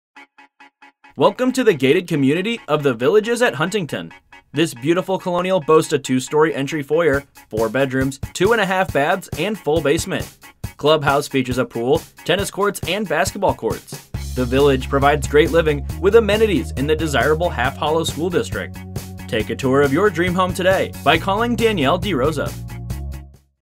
5 Dylan Place Melville, NY | ColdwellBankerHomes.com
5 Dylan Place, Melville, NY

Welcome to the gated community of the Villages at Huntington! Beautiful colonial with two-story entry foyer, four bedrooms, two and one-half baths, and full-finished basement. Clubhouse features a pool, tennis courts and basketball courts. Located in the desirable half hollow school district. The Vi